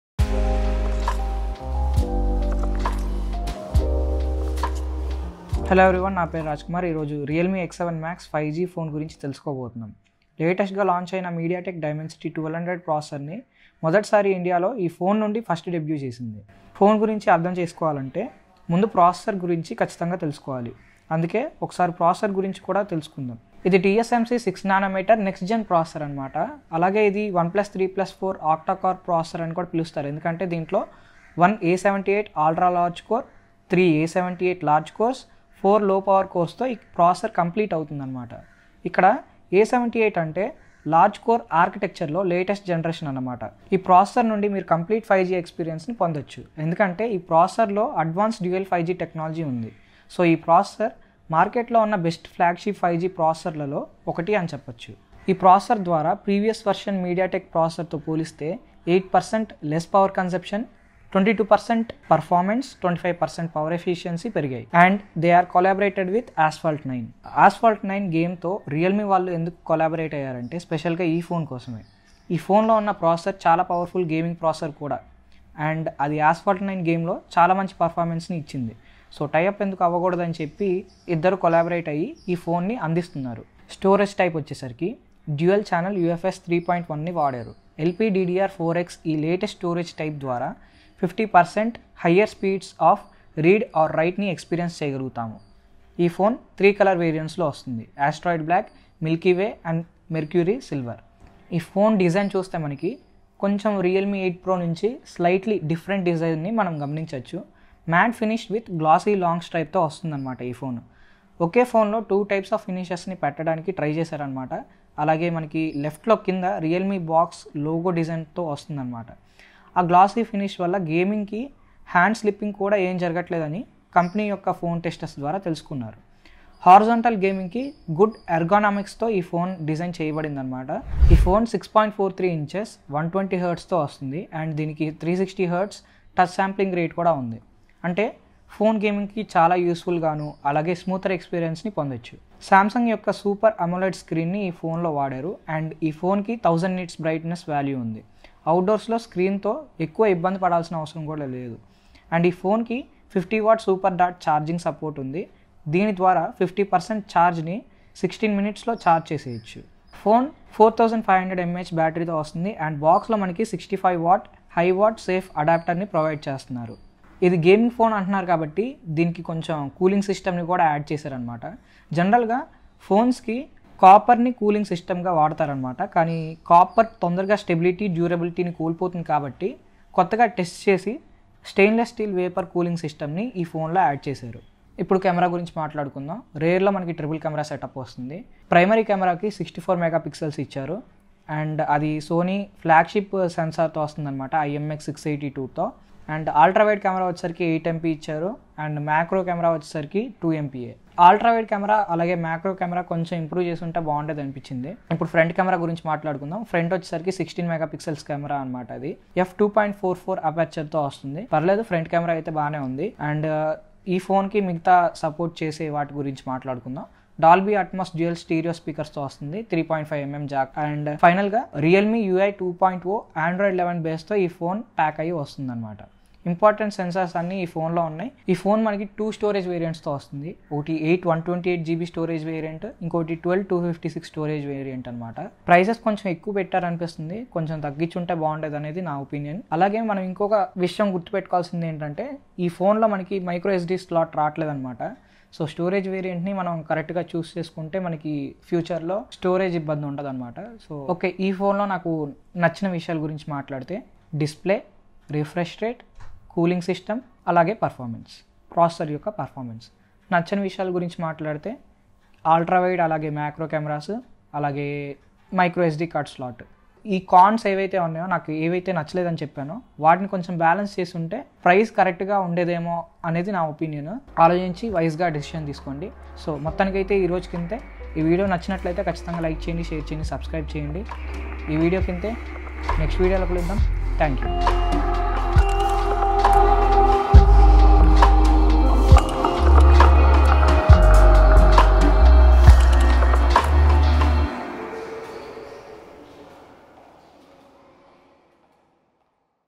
Should you buy Realme x7 max.? | Telugu | Is it worth?
Price:
8/128gb --  26,999/-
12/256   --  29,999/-

This phone is a mid-range entry level gaming smartphone which gives you amazing performance.. while gaming.